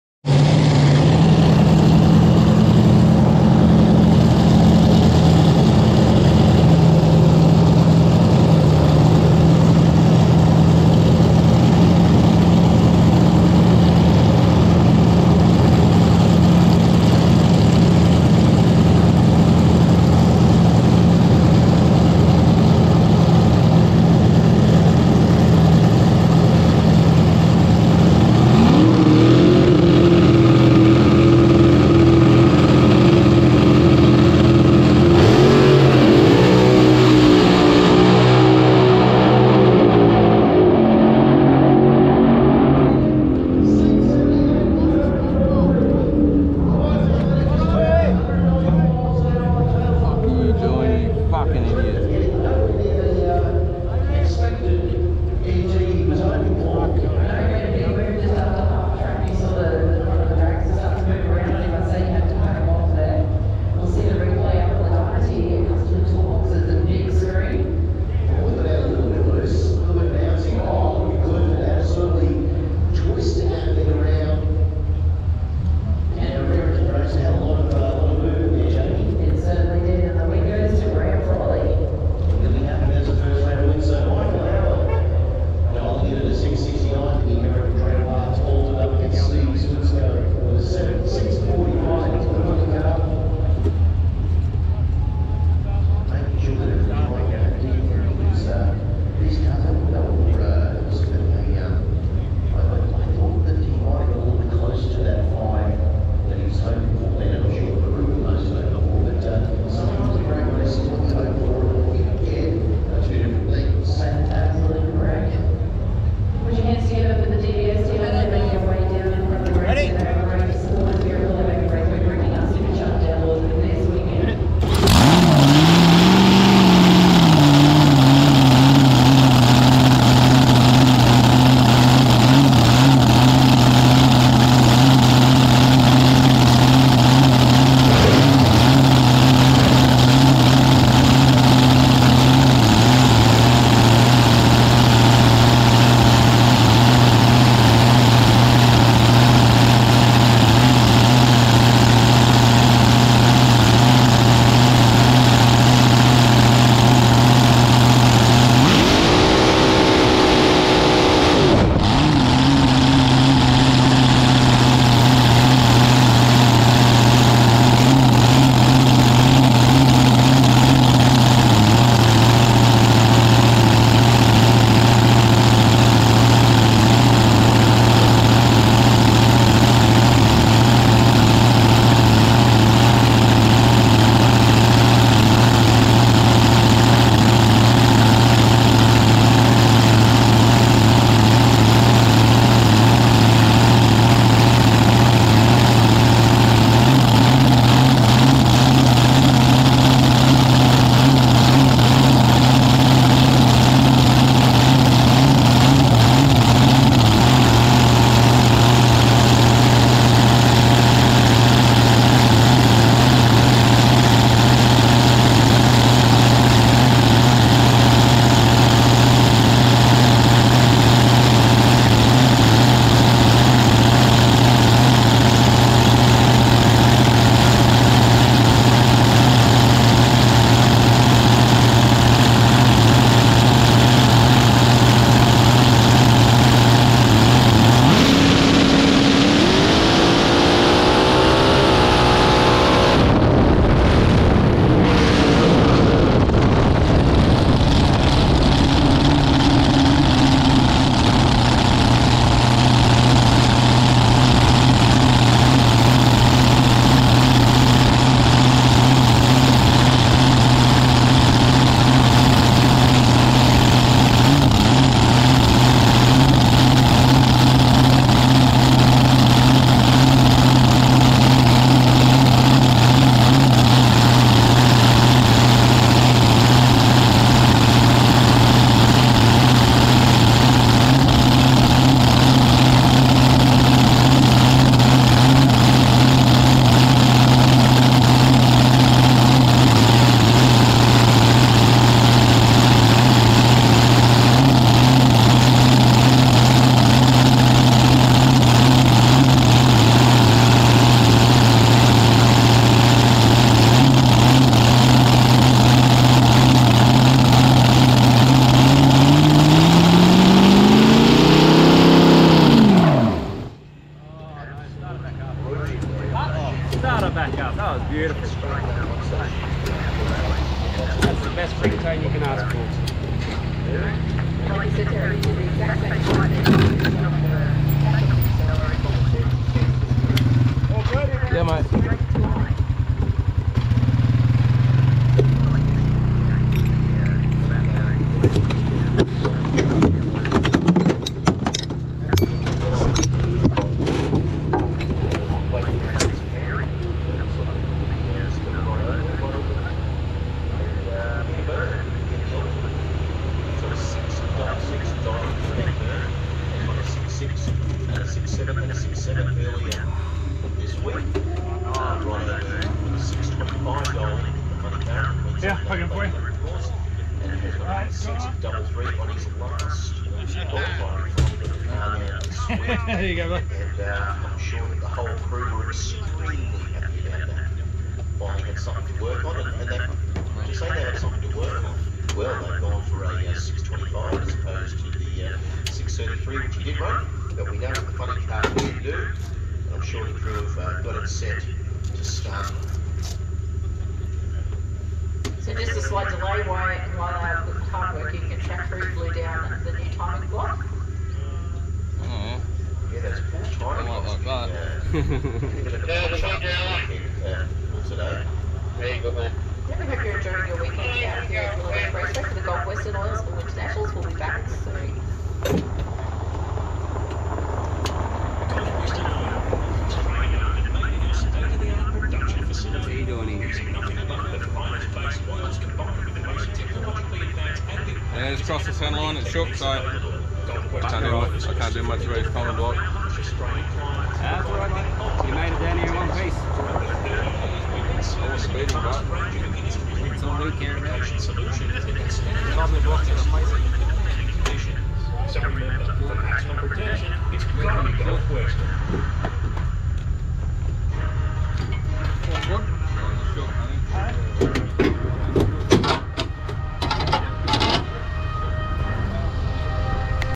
Michael Howard First round at 2021 Winternationals
First round at 2021 Winternationals ... a bit eager on the tree.
Now get ready for next year.